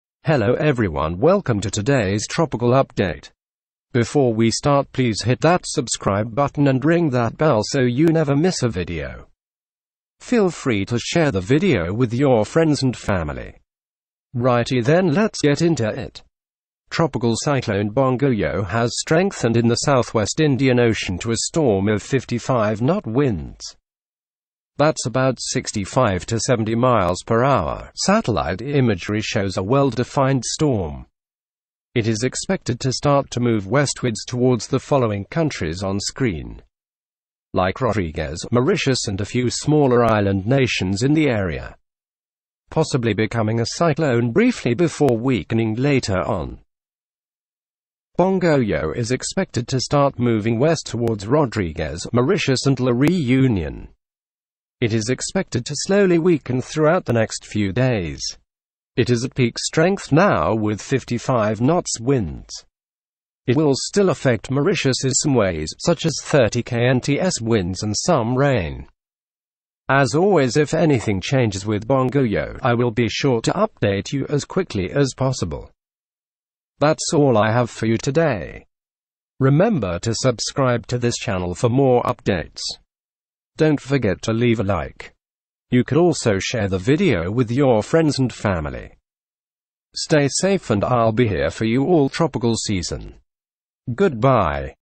Tropical Cyclone Bongoyo at Peak Strength! Will it Affect Mauritius & Rodrigues?
Tropical Cyclone Bongoyo is at peak strength as it starts moving west towards Mauritius, Rodrigues and La Reunion.
Is there still a chance it can strengthen into a Cyclone? Watch this video to find out the details if this system and to see if it will affect you.